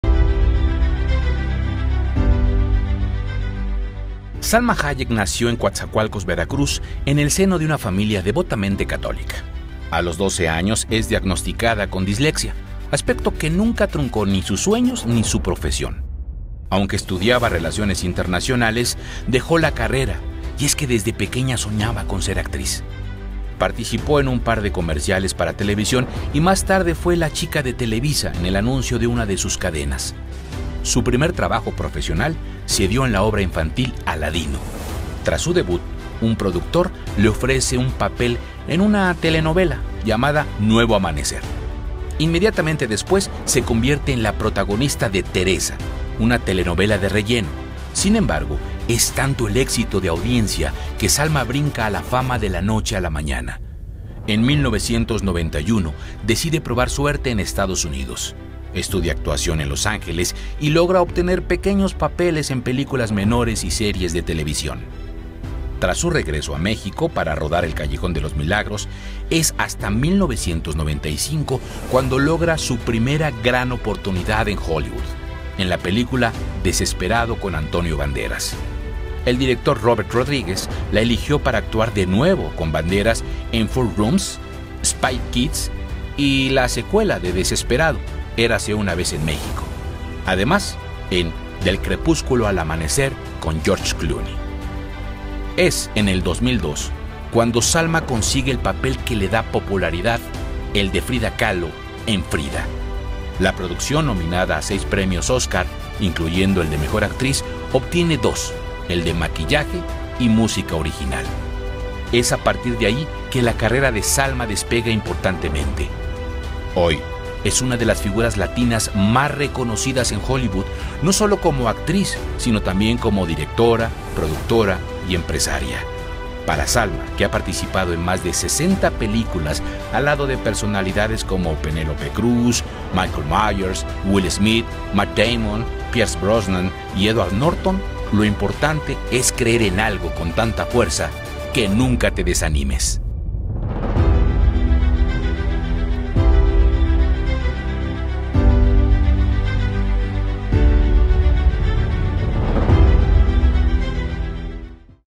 Salma Hayek, la mexicana que conquista la industria del cine en Hollywood
MexicanosdePoca | 🇲🇽 La historia de Salma nos recuerda que el talento, la pasión y la determinación pueden abrir oportunidades ilimitadas. ¡Conoce su historia!